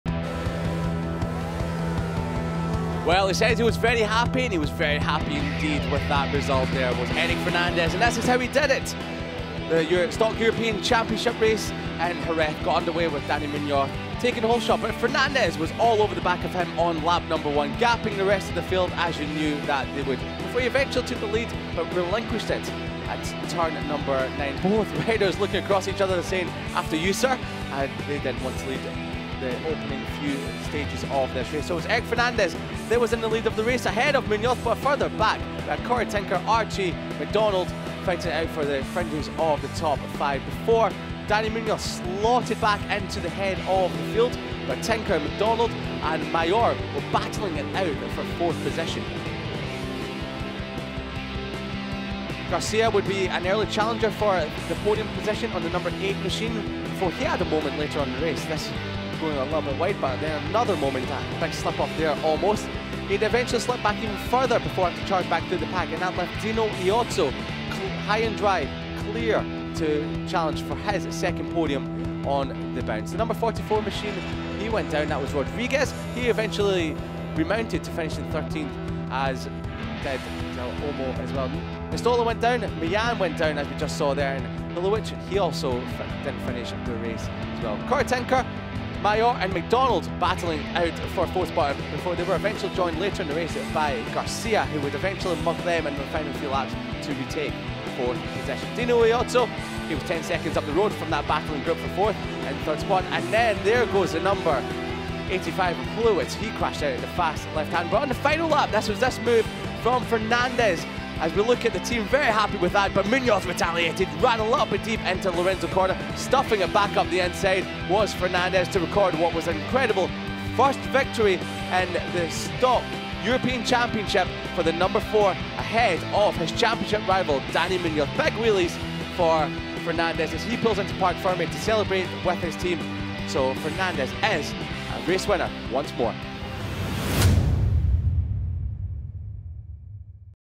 🏁 Stock ECh Race Highlights | Round 3 Jerez | 2023 Finetwork FIM JuniorGP™ World Championship
Fernandez halts Muñoz's perfect record! 🥇

Watch the best moments of the third race in history in this category 🏍️